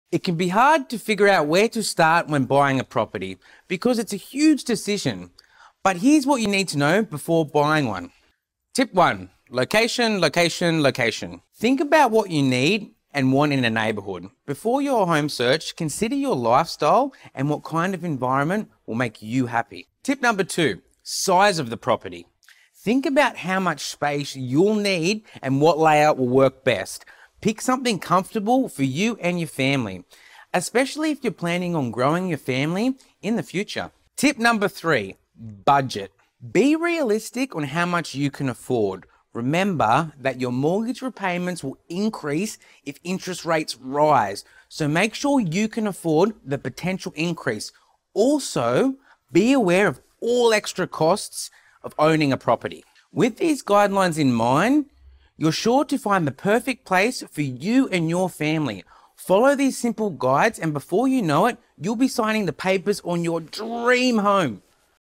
3 Tips in Choosing the Right Property | Australian Property Investing
Finding the ideal house is always challenging, especially under the strain of a significant financial commitment. So, when choosing a property, keep it in mind.

 Location – Think about the surroundings and neighborhood; is it somewhere you can see yourself living in the future!
 Size of the property – Analyze the kind of space you require now and in the future.
 Budget – Be realistic about your financial situation and locate a location that fits your budget.

You'll find the ideal property for you if you keep these items in mind!